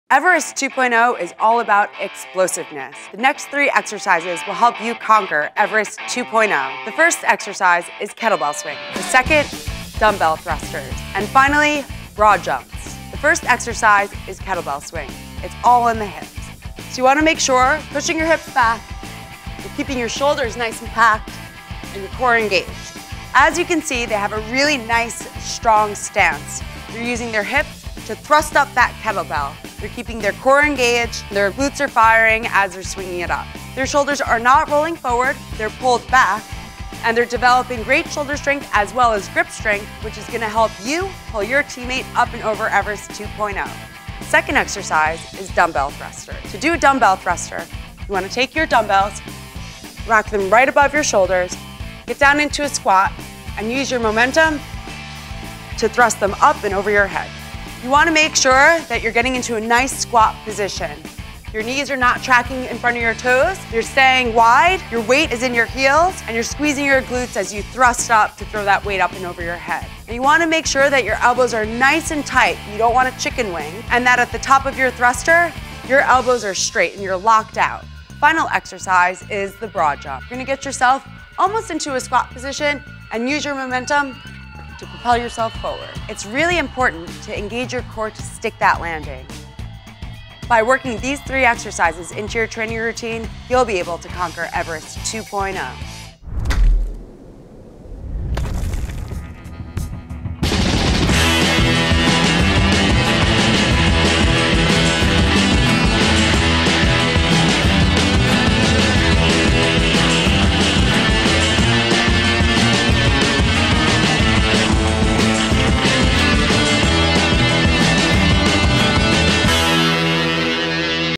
How to Train for the Everest 2.0 Obstacle | Tough Mudder Training
Train for the Everest 2.0 obstacle using this at home workout. You'll do dumbbell squats, kettlebell swings, and squat jumps in this full body workout.

This obstacle course training video is an at home workout that teaches you how to train for Tough Mudder's Everest 2.0 obstacle.